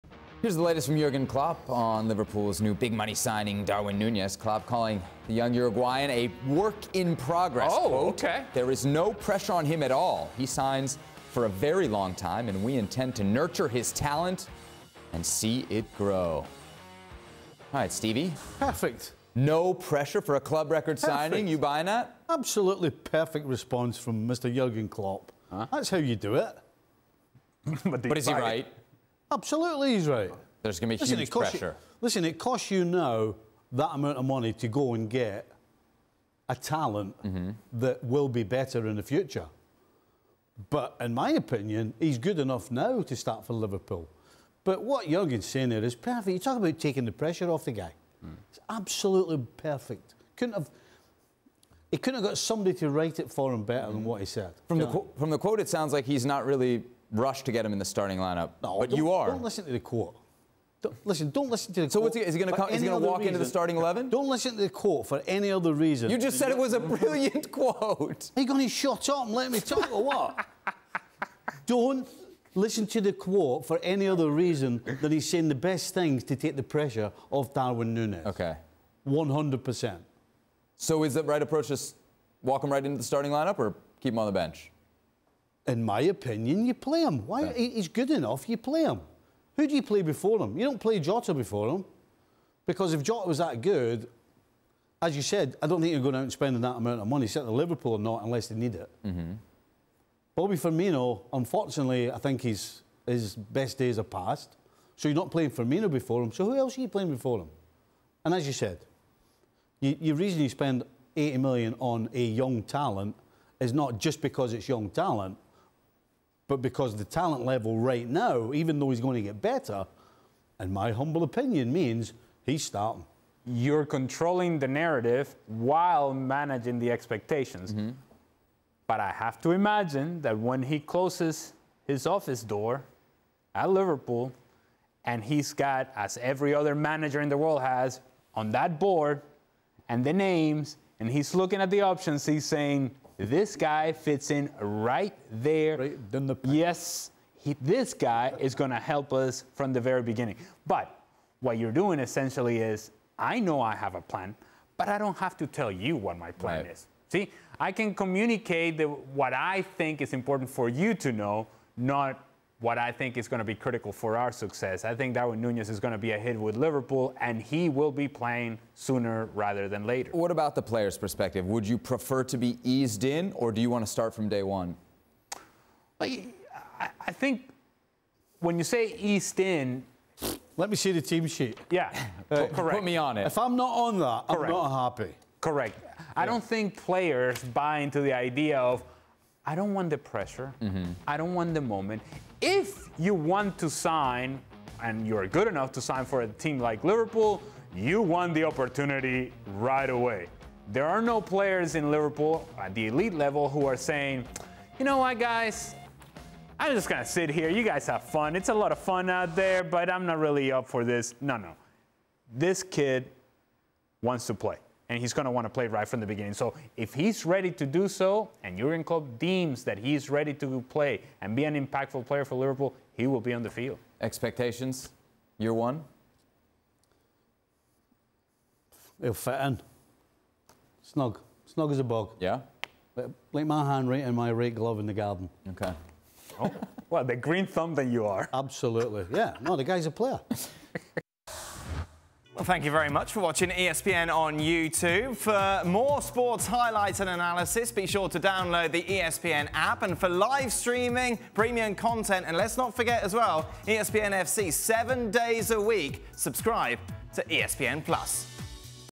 No pressure on Darwin Nunez? ‘PERFECT RESPONSE’ from Klopp! - Steve Nicol | ESPN FC
The ESPN FC crew react to Jurgen Klopp’s comments about new signing Darwin Nunez and Steve Nicol completely loves the way Klopp has addressed the pressure on the young Uruguayan.